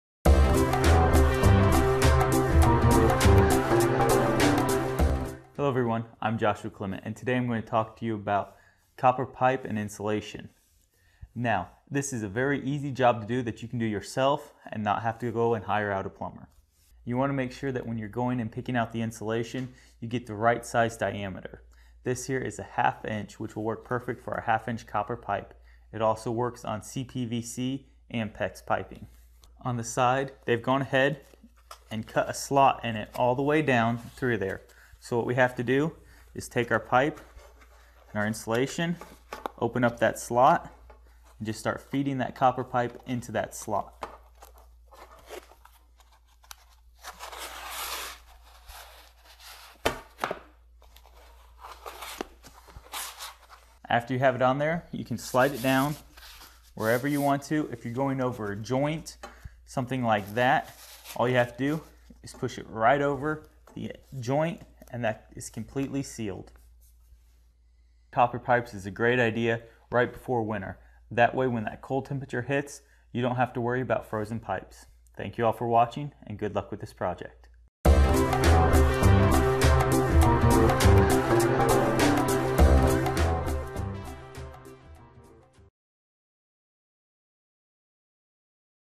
How to Install Copper Pipe Insulation
How to Install Copper Pipe Insulation. Part of the series: Copper Pipes & Fittings. Installing copper pipe insulation is something that you have to do very carefully to help make sure that everything works as it should. Install copper pipe insulation with help from a foreman for Lighty Contractors in this free video clip.